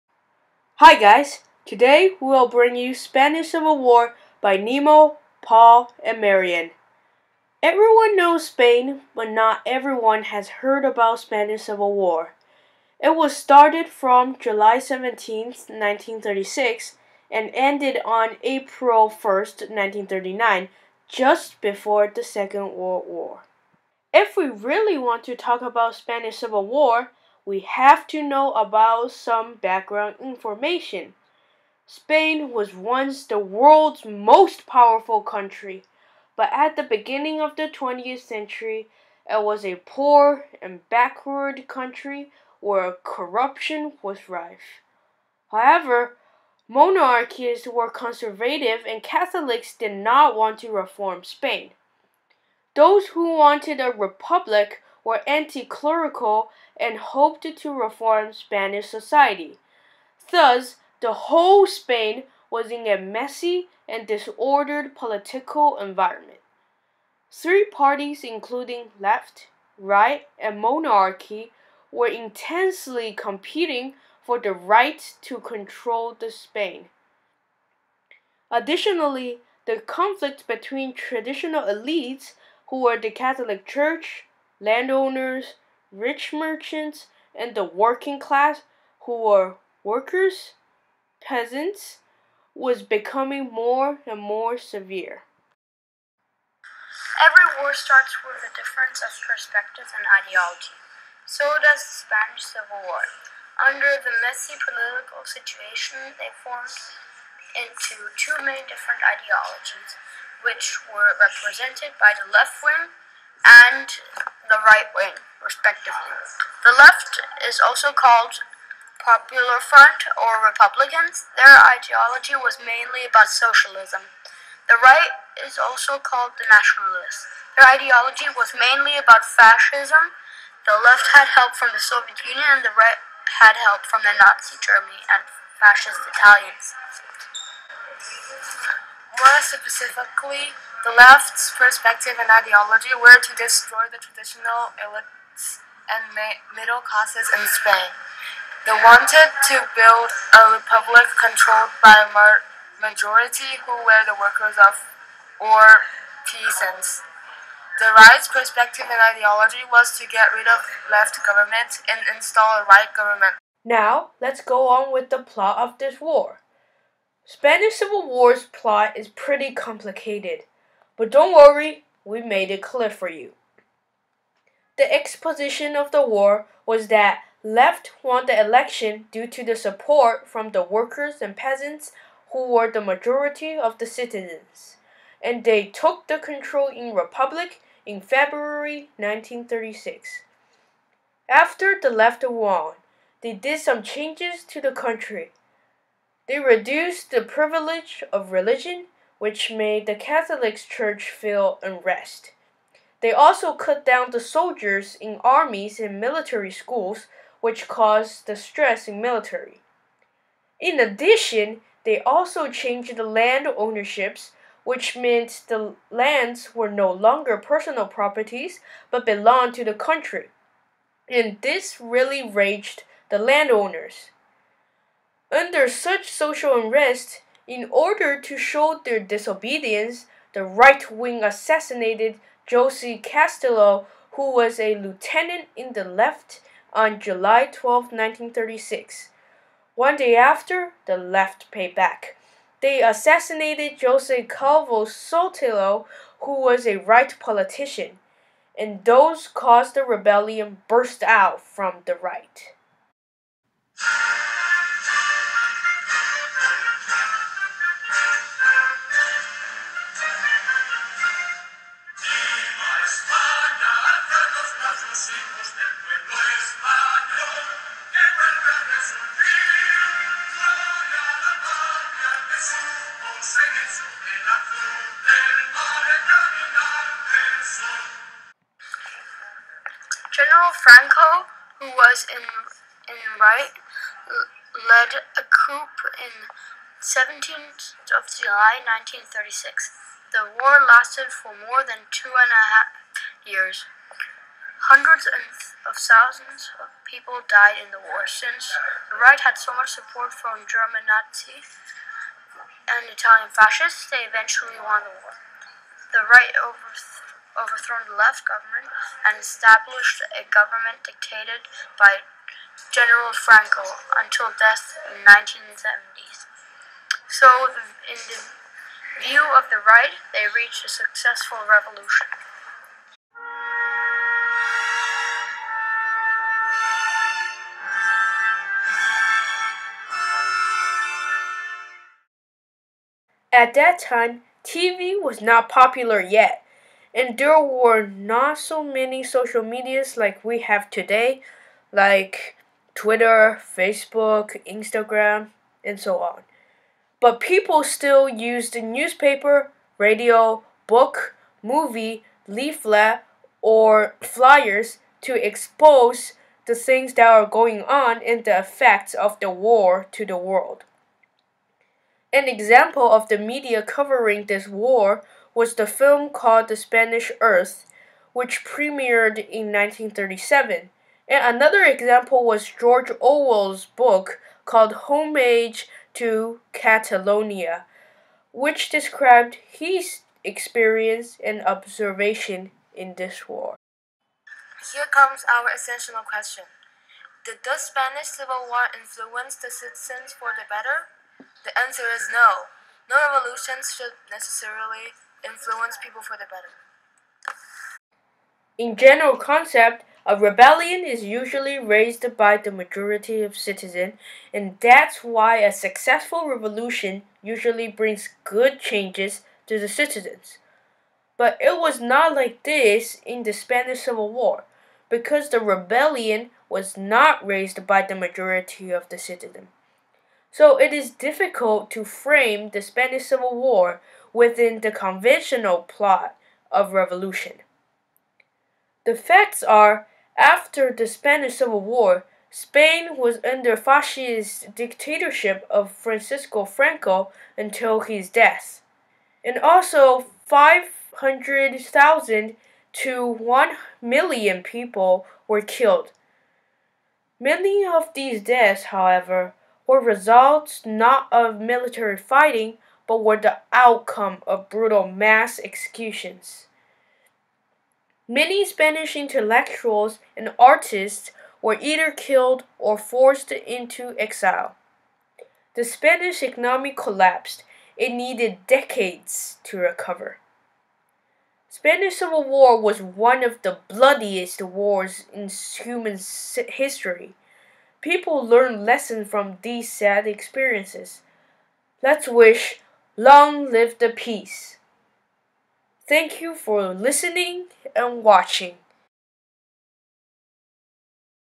Spanish Civil War
This video is about brief info of Spanish Civil War.  Also, this vid explained the process of change in Spanish Civil War.  (P.S. Including Essential Question)